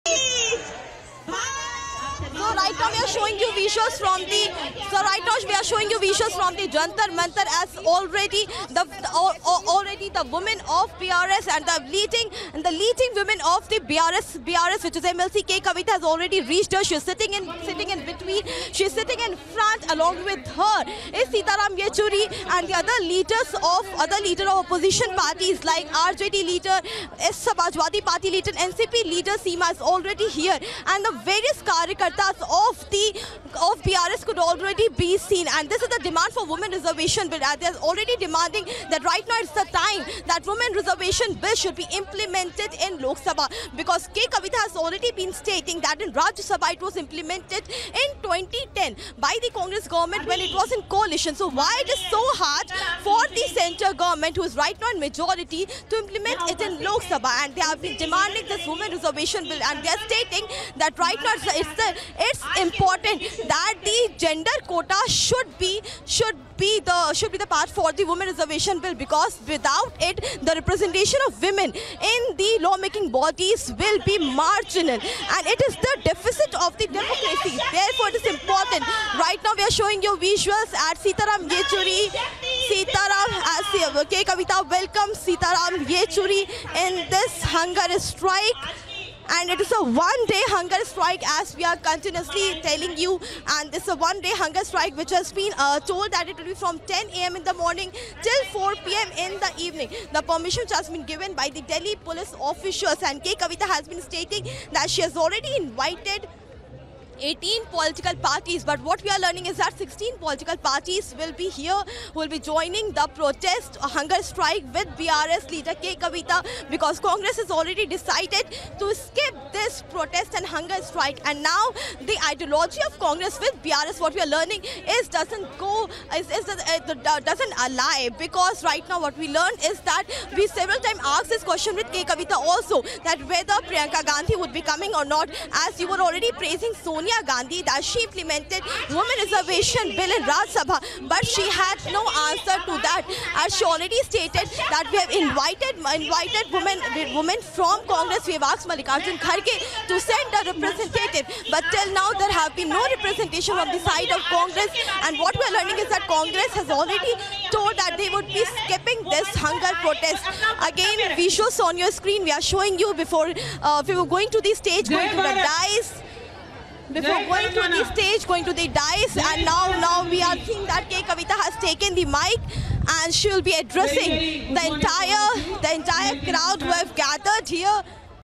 K Kavitha Leads One-Day Hunger Strike In Delhi To Seek The Introduction Of Women's Reservation Bill
Bharat Rashtra Samithi MLC K Kavitha leads one-day hunger strike in the National Capital to seek the introduction of the Women's Reservation Bill in the current Budget session of Parliament.

Reporter: Harsha Chandwani